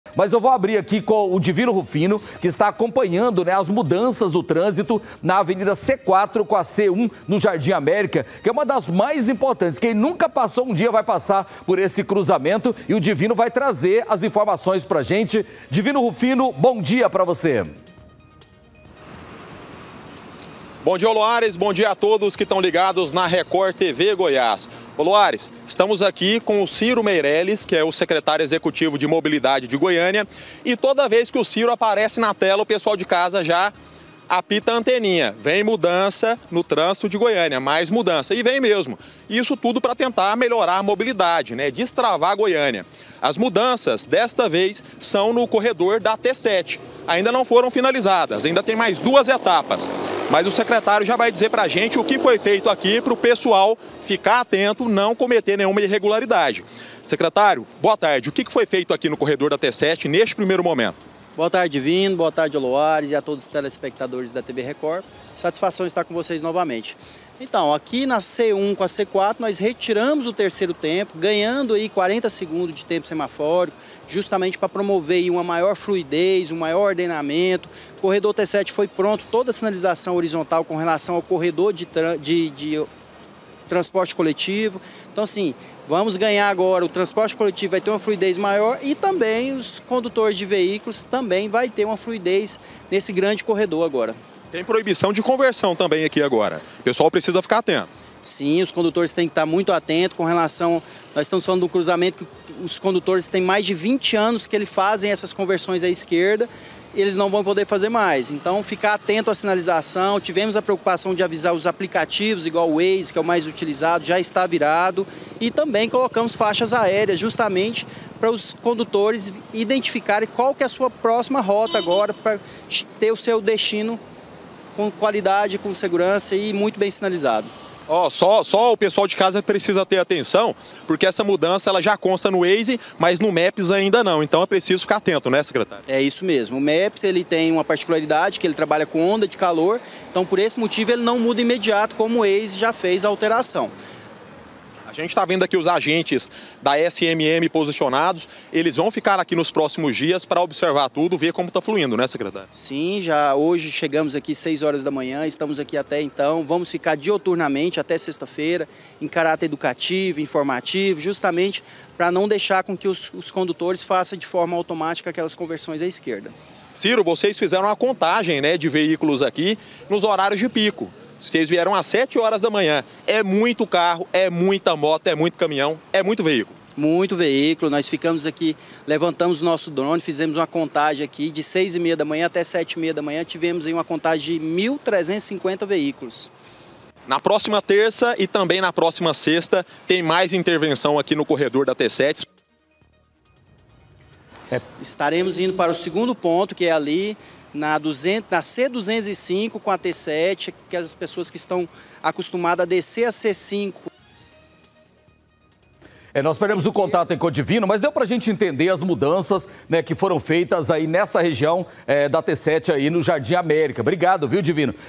CORREDOR DA T-7: MUDANÇAS NO TRÂNSITO NO JARDIM AMÉRICA
DIVINO RUFINO CHEGA PRA FALAR SOBRE AS MUDANÇAS NO TRÂNSITO NA AVENIDA C-4 COM AVENIDA C-1, NO JARDIM AMÉRICA.